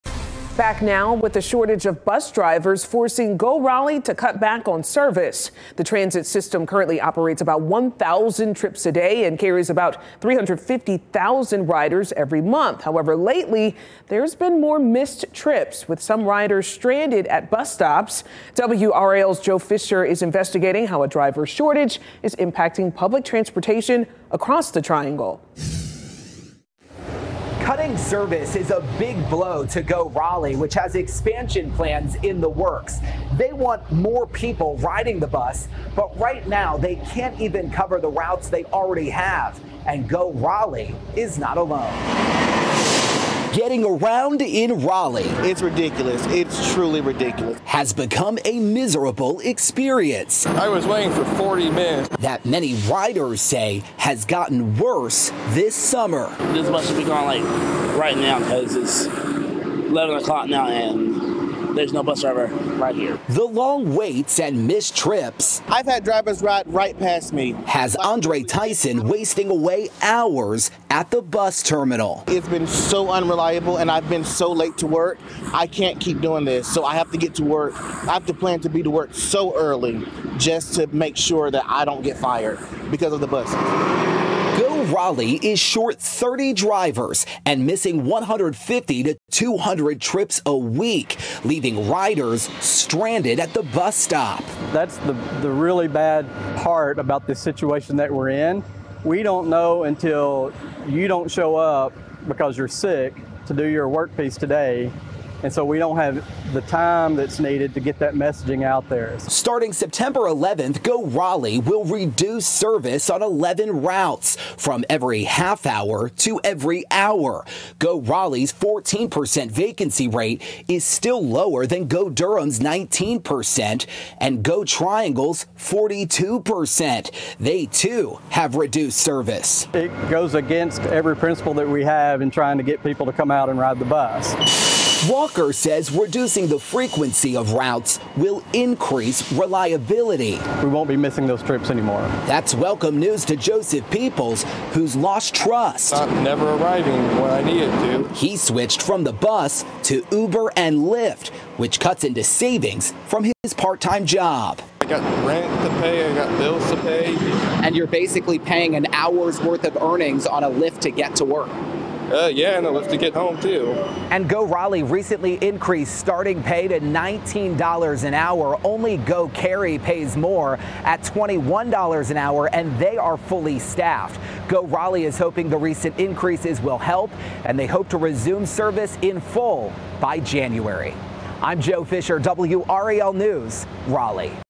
GoRaleigh is reducing service starting Sept. 11; Driver shortage faults expanded services
The transit system currently operates about 1,000 trips a day and hauls about 350,000 riders every month. But lately, there's been more missed trips with the riders stranded at the bus stop.

They want more people riding the bus. But right now, they can’t even keep up with the routes they already have.

"A lot of the time, you want to be angry with the bus driver, but you can't be angry with the bus driver because you know they are short-staffed because my job is also incredibly short staffed," said Tyson.